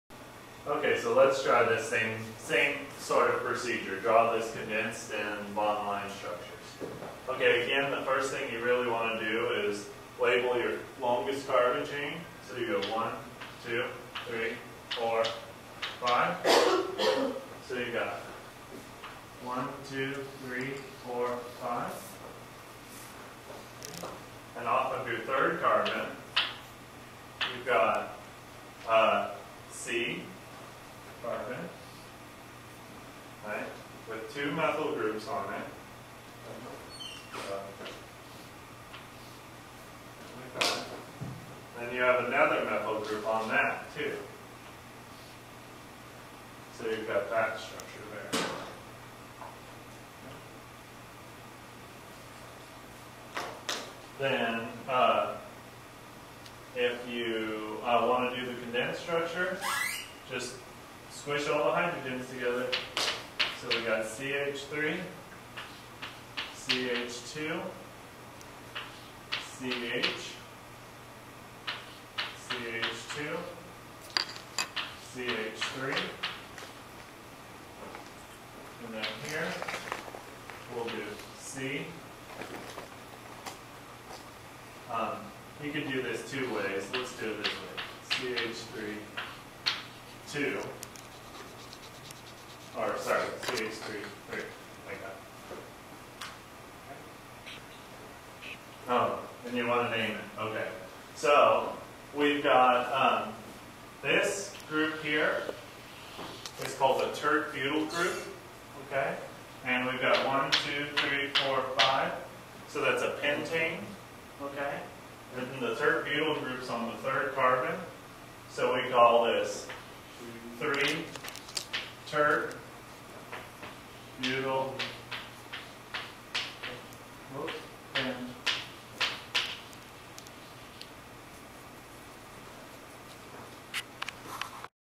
Draw Condensed and Bond Line Structure 001
Draw the condensed and bond-line formula for the following structure (3-ethyl-2,2-dimethylpentane).